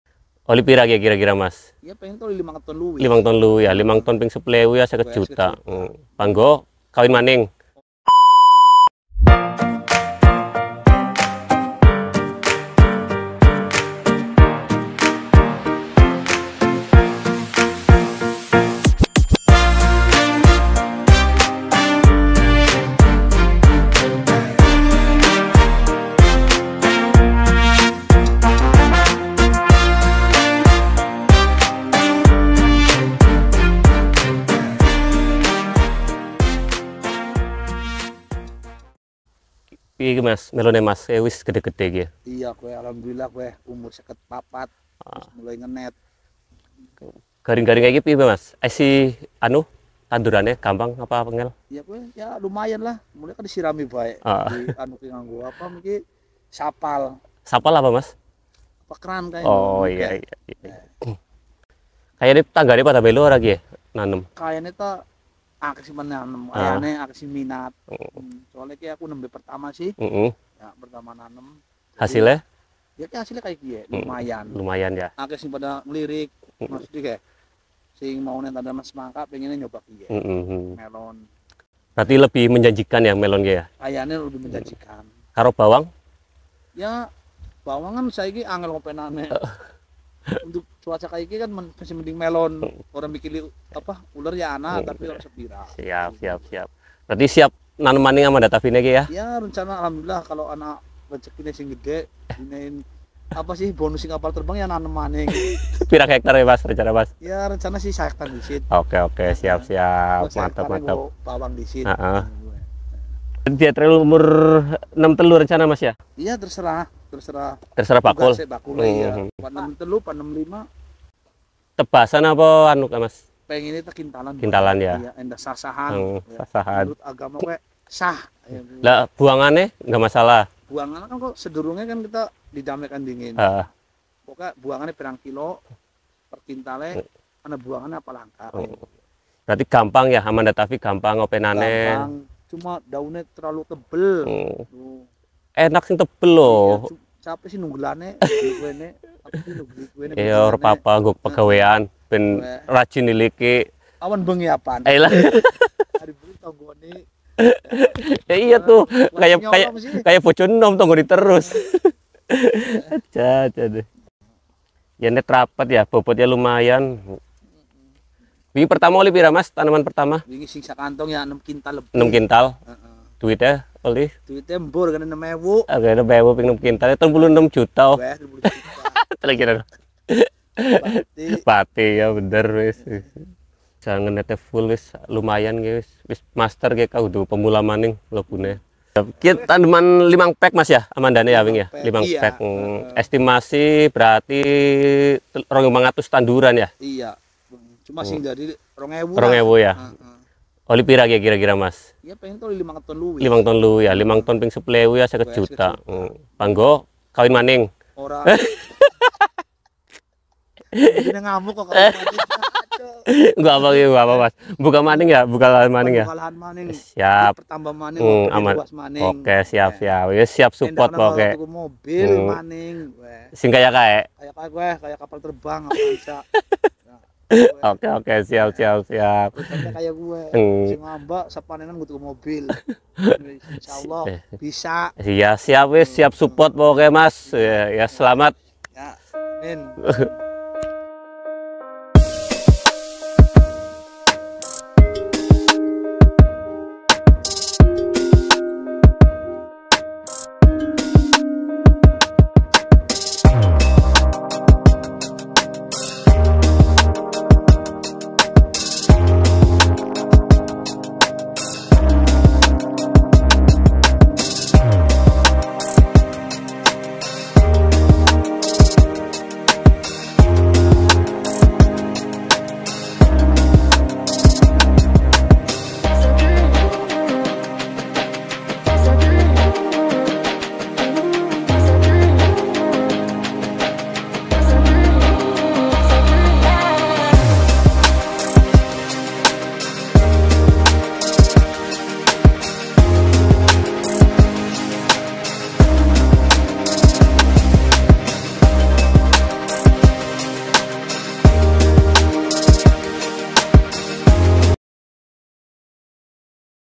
KOCAK ‼️OBROLAN PETANI NGAPAK TANAM MELON AMANDA TAVI
MELON AMANDA TAVI BIKIN PETANI PEMULA PENASARAN ‼️ SEMPAT HILANG DI PASARAN ⁉️

Disaat petani mengalami kegagalan, tak pernah terbersit baginya untuk menyerah.
Mereka akan berinovasi kembali untuk mendapatkan hasil yang tinggi...

Kali ini kita berkunjung ke lahan melon amanda tavi milik pak wawan di desa karangreja, kecamatan tanjung, brebes..

Banting setir dari petani jagung manis, ia memantapkan menjadi petani melon Amanda sejak tahun ini..

Baginya melon amanda sangat ramah bagi petani pemula sekalipun lahan yang ia gunakan mengalami kekeringan..

Seperti apa cerita selengkapnya?